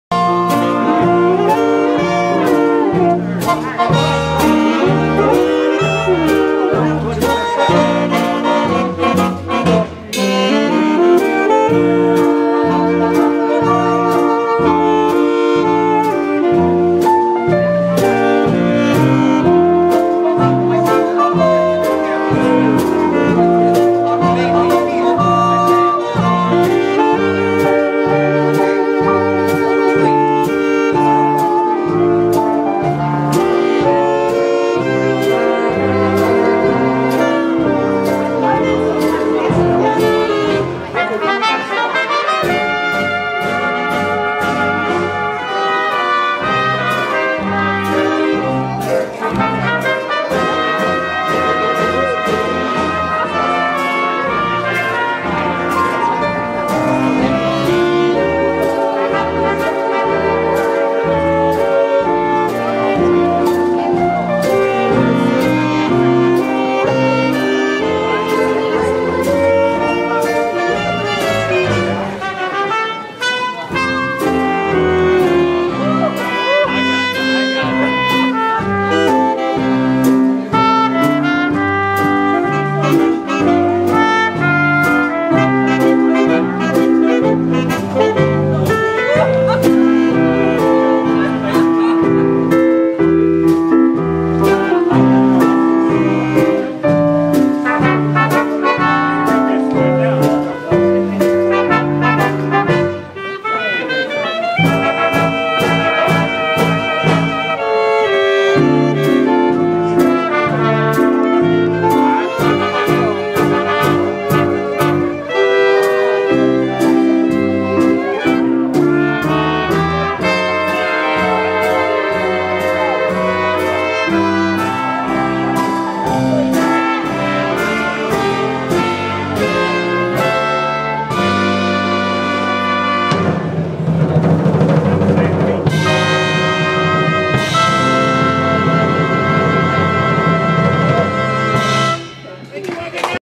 Teddy Lee Orchestra (2017) -
Ballroom Dancing @ Glendora Banquets -10225 S. Harlem Ave. - Chicago Ridge, Illinois 9-3-2017